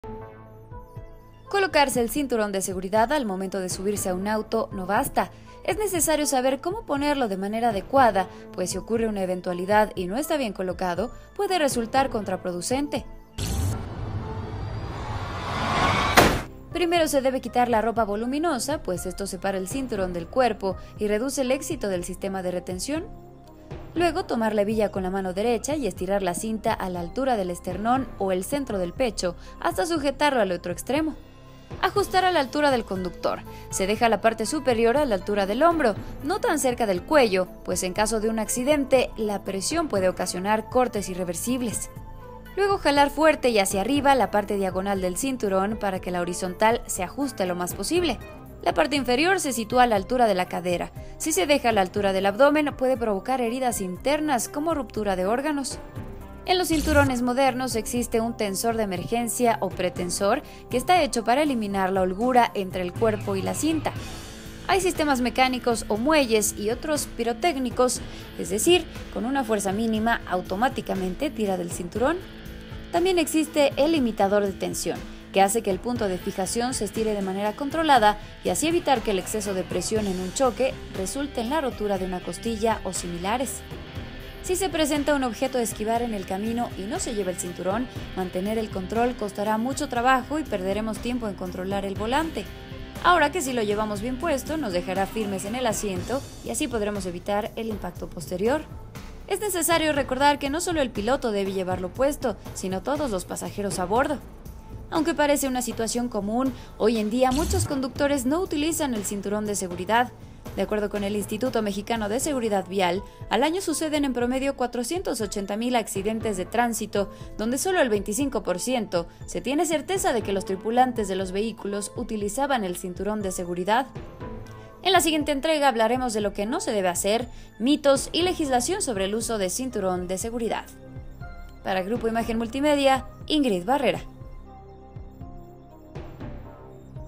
Razones por las que debes de utilizar el cinturón de seguridad
Te presentamos las razones por las que debes de utilizar el cinturón de seguridad en el automovil y también la forma correcta de usarlo. Recuerda que te puede salvar la vida en caso de accidente.